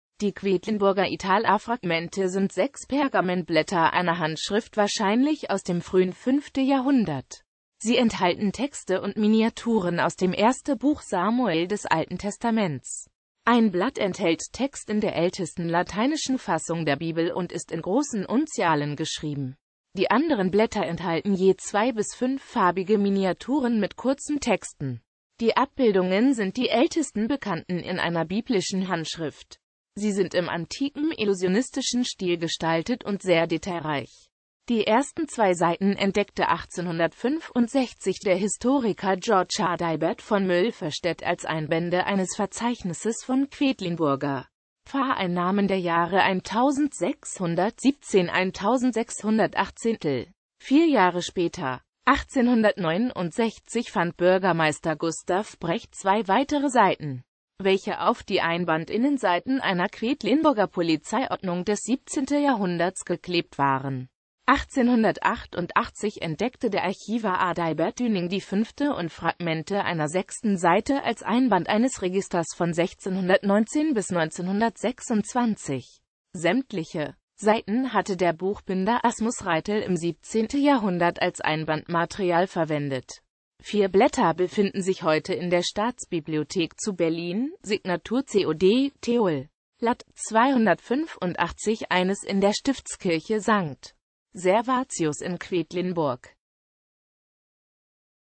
Quedlinburger Italafragmente
Vielen Dank für Ihre Unterstützung:

Die Quedlinburger Italafragmente sind sechs Pergamentblätter einer Handschrift wahrscheinlich aus dem frühen 5.Jahrhundert.Sie enthalten Texte und Miniaturen aus dem 1.Buch Samuel des Alten Testaments .